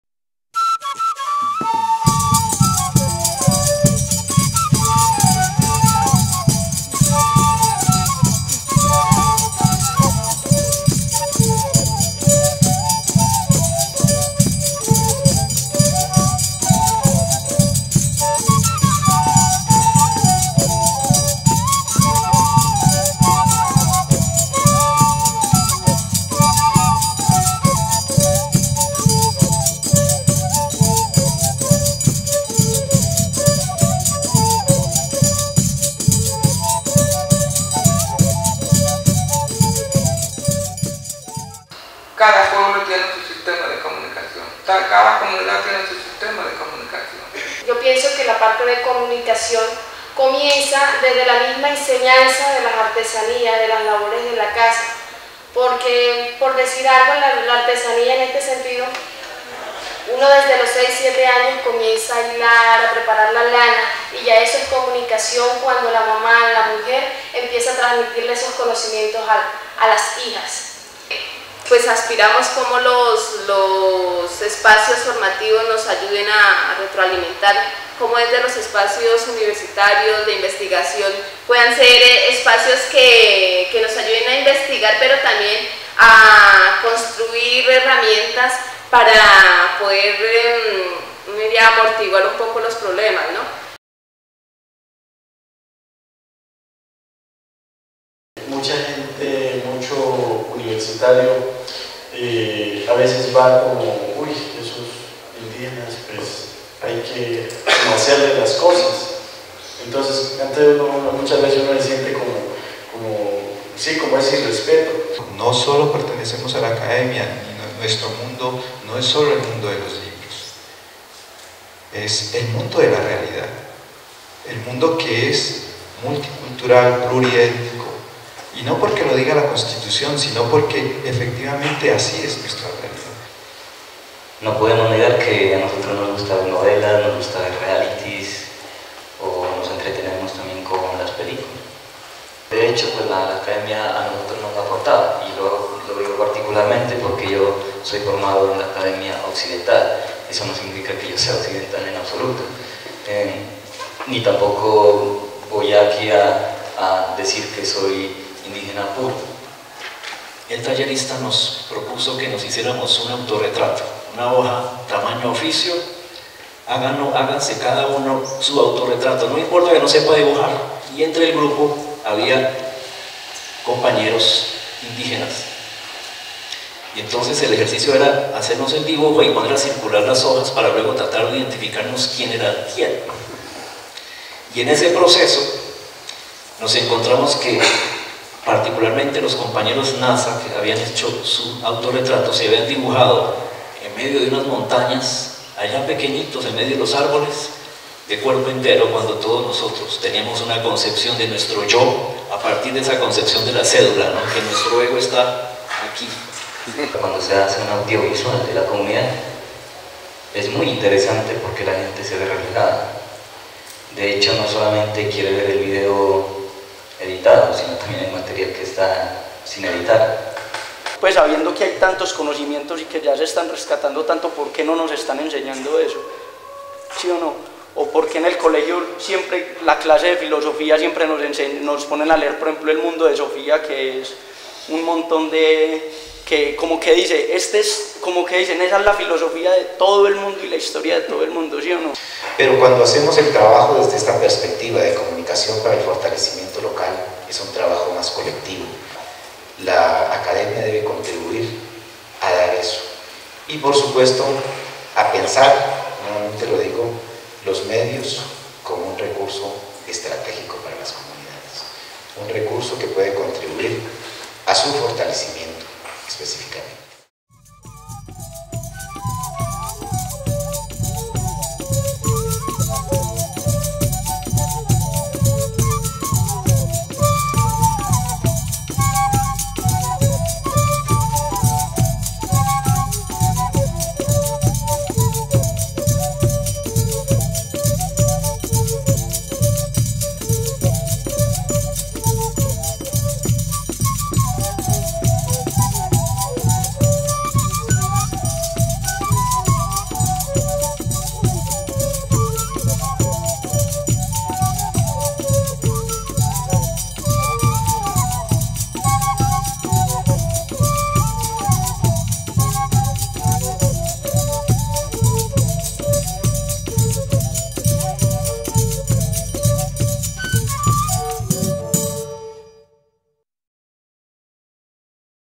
DAUPARA 2010
Un vistazo a las actividades que hicieron parte de Daupará 2010 - Muestra de cine y Video Indígena en Colombia, que se realizó en Popayán